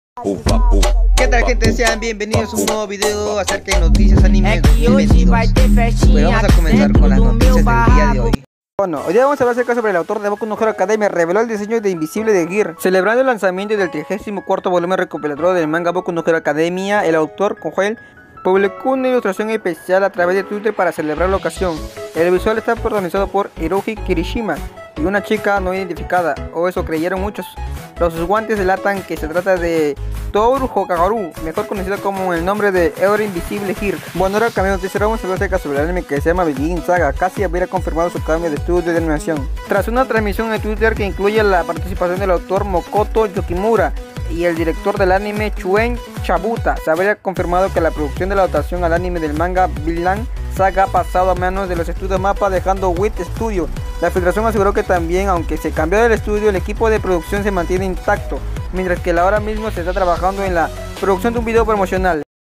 El autor de Boku no Hero Academia reveló el diseño de Invisible Girl
Celebrando el lanzamiento del trigésimo cuarto volumen recopilatorio del manga Boku no Hero Academia (My Hero Academia), el autor Kohei Horikoshi publicó una ilustración especial a través de Twitter para celebrar la ocasión. El visual está protagonizado por Eijirou Kirishima y una chica no identificada… ¡o eso creyeron muchos! Los guantes delatan que se trata de Tooru Hagakure, mejor conocida con el nombre de héroe Invisible Girl.

Cabe recordar que el capítulo 337 de la obra, publicado en diciembre de 2021, se reveló finalmente el rostro de Invisible Girl durante un gran destello, sumado a la revelación del traidor dentro del grupo de héroes de la Academia UA. Si bien en aquel entonces solo fue el rostro, fue suficiente para inspirar una variedad de fan-arts por parte de la comunidad de artistas. Aún así, hay muchas cosas que todavía no son claras sobre su diseño, como el color de su cabello.